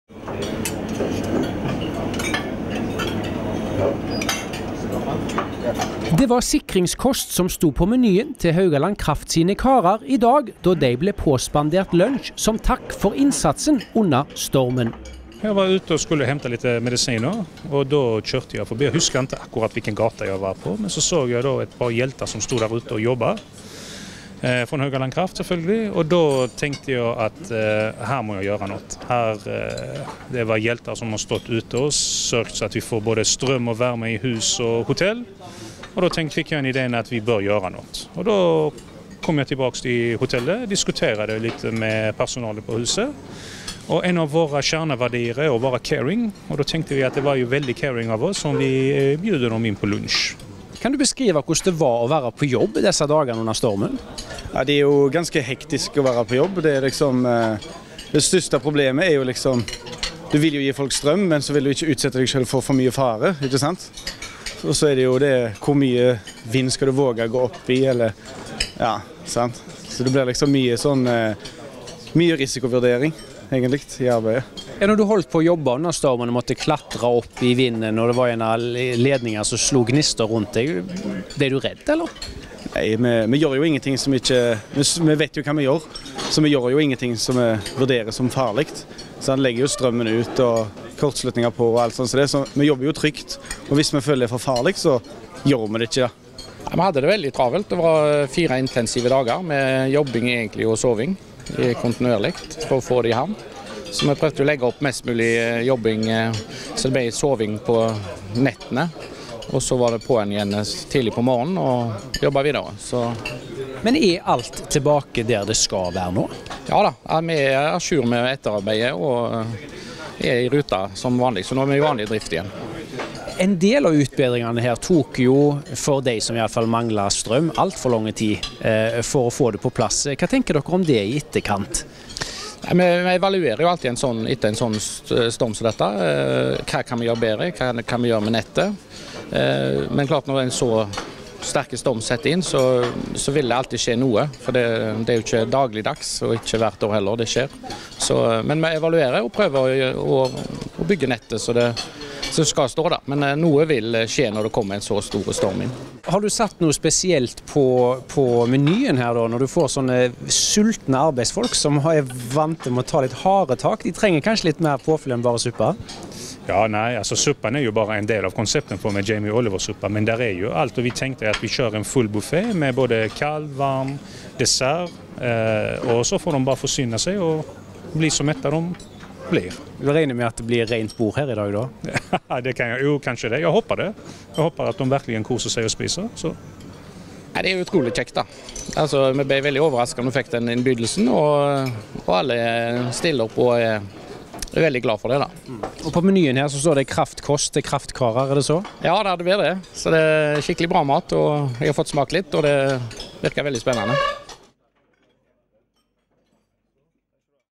Kraftkost til kraftkarer
Som takk for innsatsen under stormen "Nina" spanderte i dag Scandic hotellet lunsj på arbeidsgjengen som stod på dag og natt under de fire dagene stormen raste.